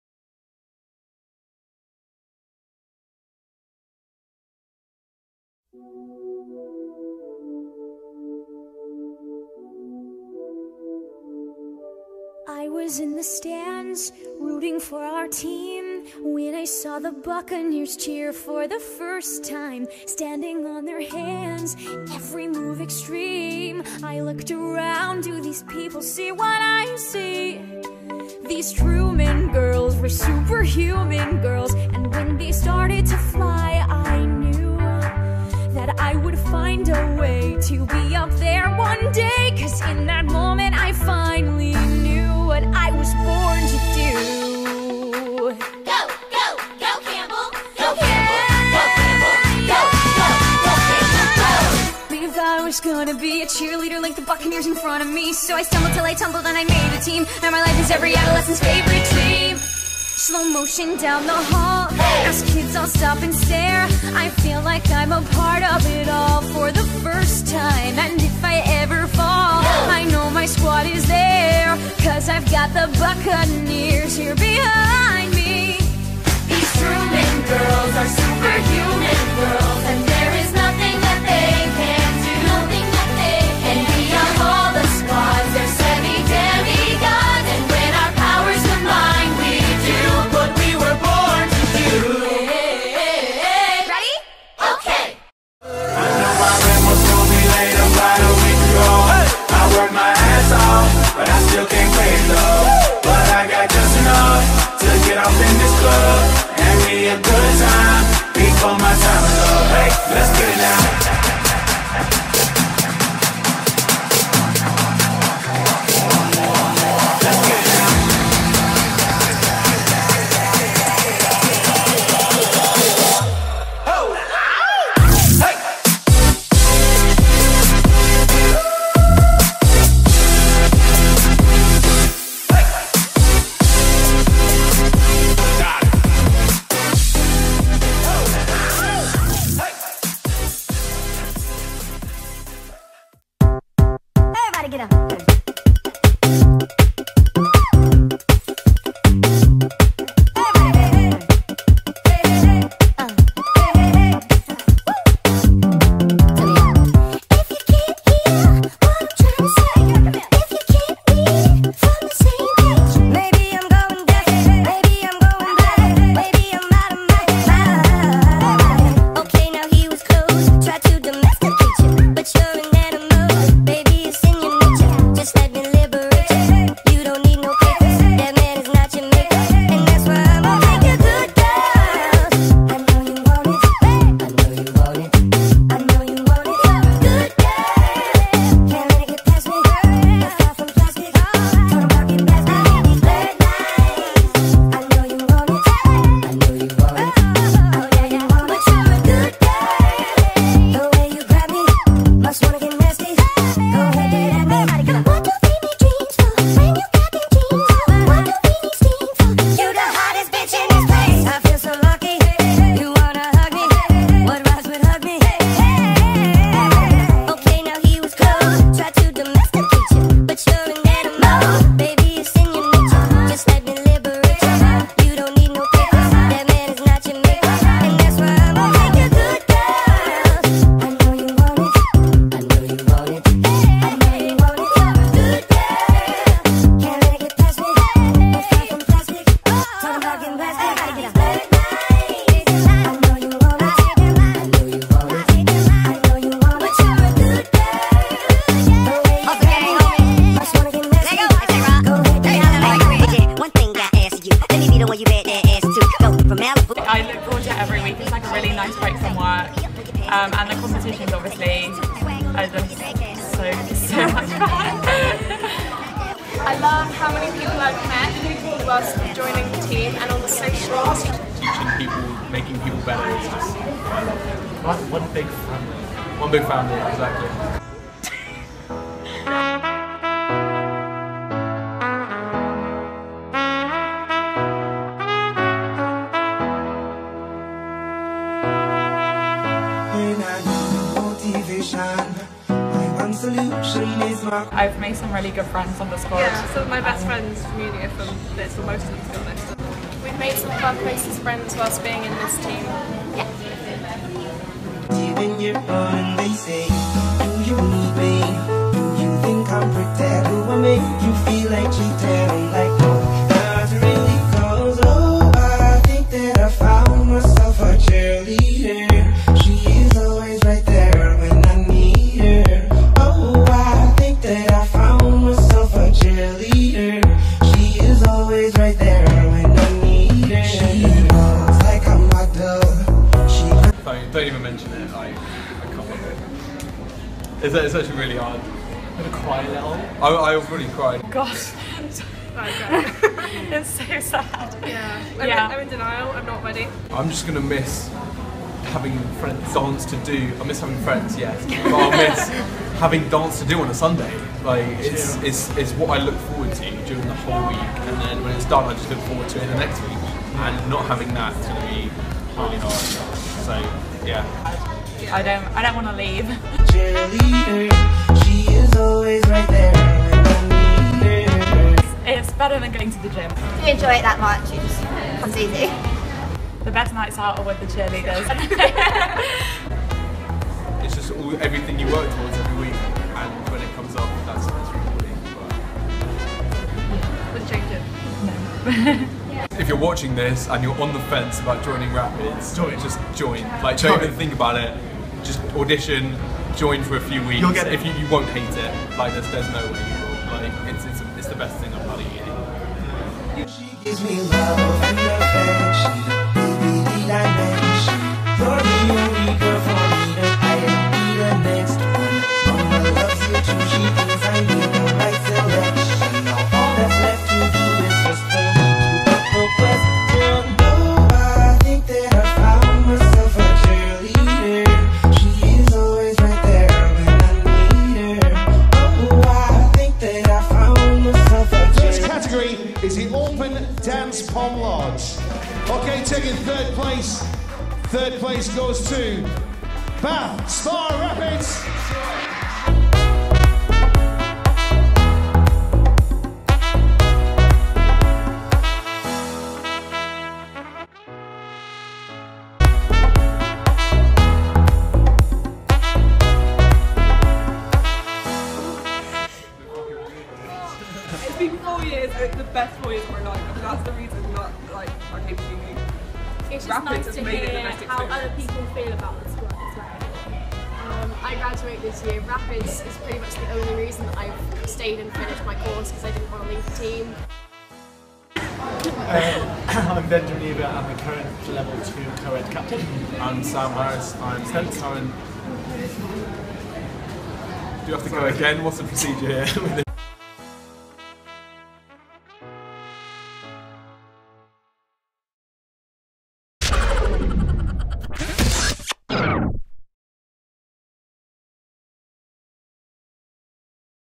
Bath Spa Rapids 2015/16 Promotion Video
* Extended Version *
The Bath Spa Rapids are a competitive cheerleading and dance society at Bath Spa University. We compete annually at BCA Nationals, Future Cheer, and Legacy.